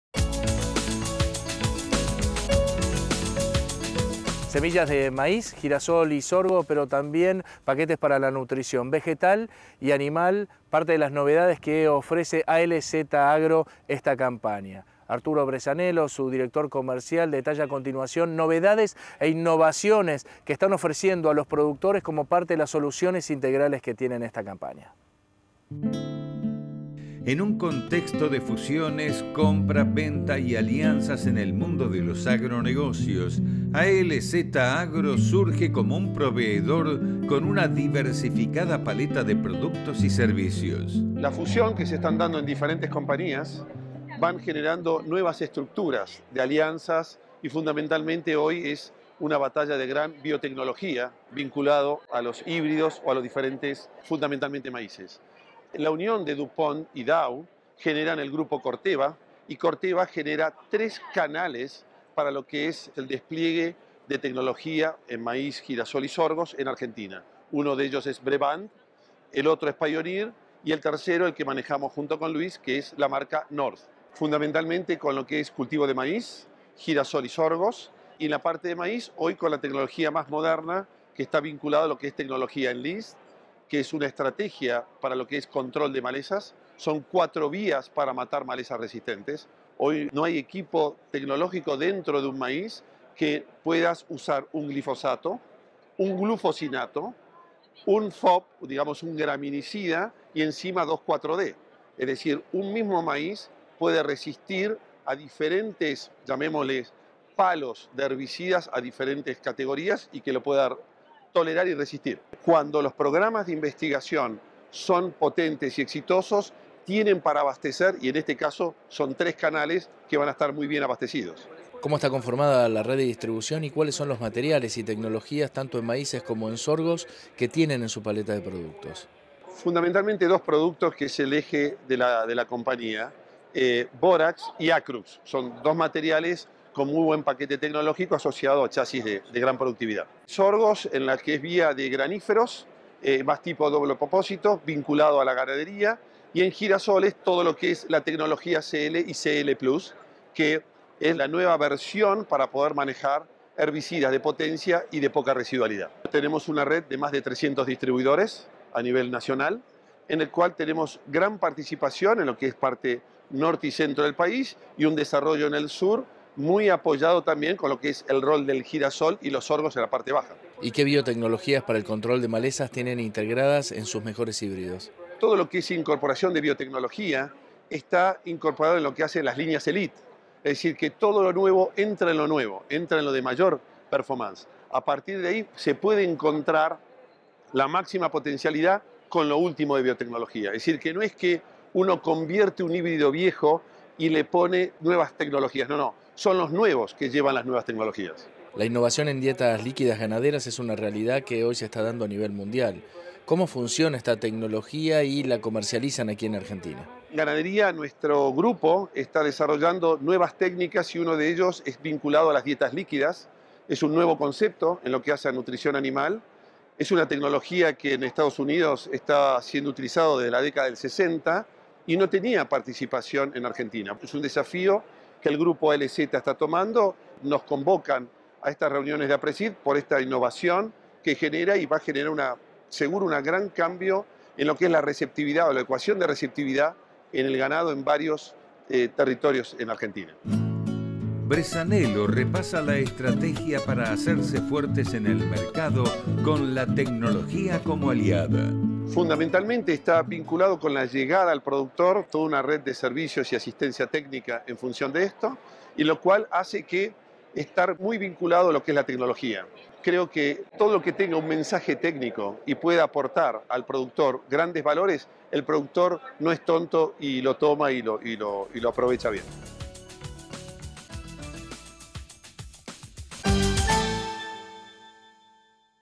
ALZ Agro: Innovaciones y soluciones para semillas, nutrientes y mercado (#787 2018-09-01)
En un contexto de fusiones, compra/venta y alianzas en el mundo de los agronegocios, ALZ Agro surge como un proveedor con una diversificada paleta de productos y servicios. Arturo Bresssanello repasa la estrategia para hacerse fuertes en el mercado con la tecnología como aliado.

- Arturo Bressanello, Director Comercial de ALZ Agro